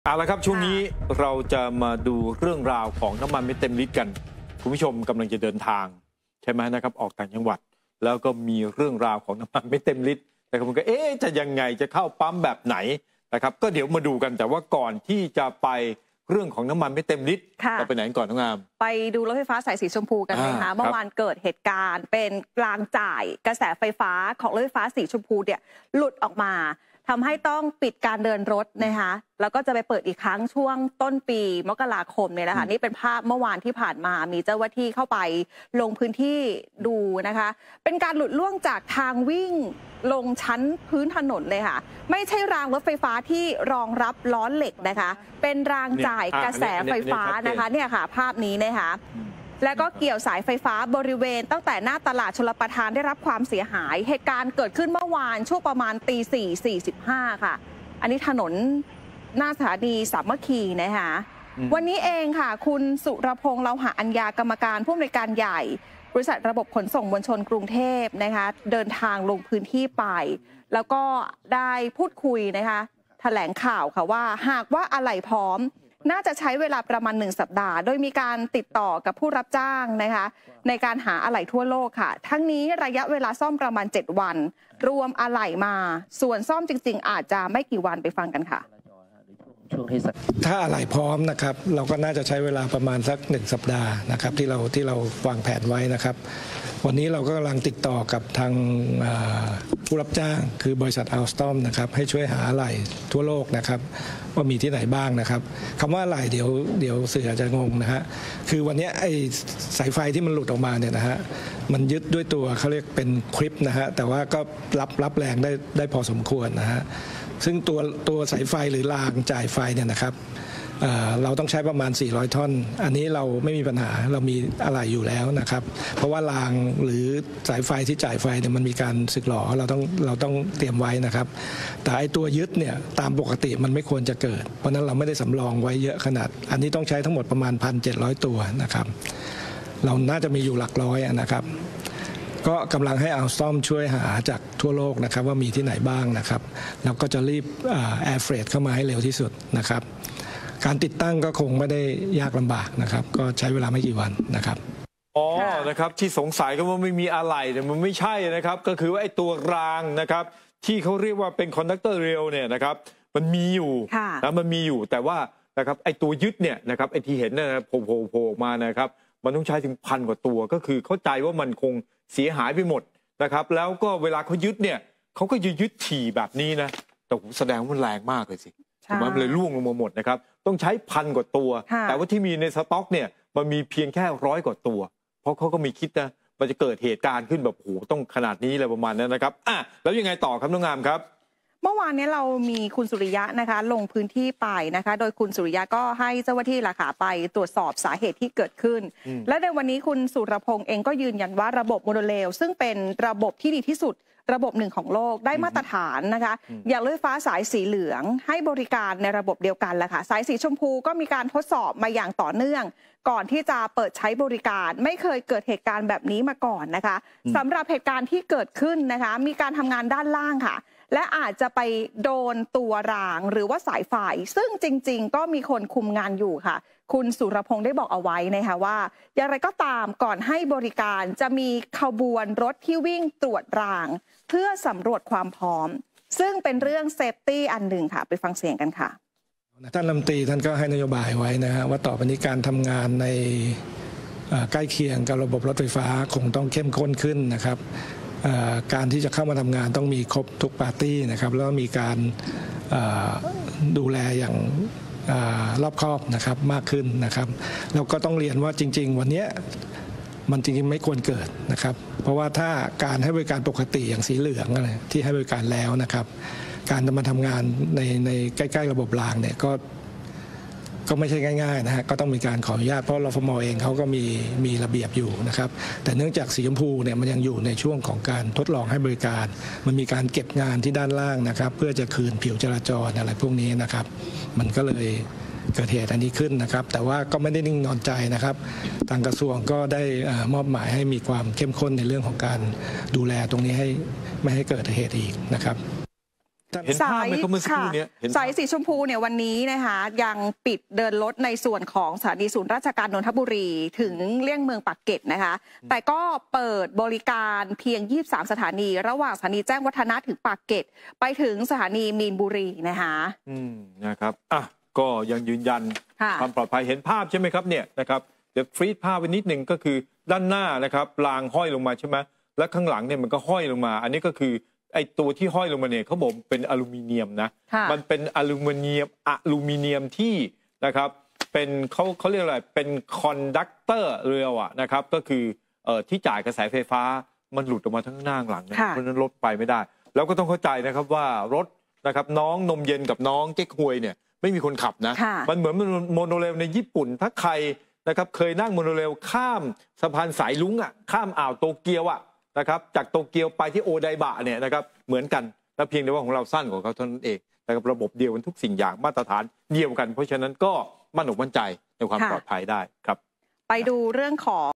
"สุรพงษ์" เร่งหาคนผิดทำรางจ่ายไฟสีชมพูหลุด สั่งตรวจสอบเข้มตลอดเส้นทาง | TOP HIGHLIGHT
"สุรพงษ์" เร่งหาคนผิดทำรางจ่ายไฟสีชมพูหลุด สั่งตรวจสอบเข้มตลอดเส้นทาง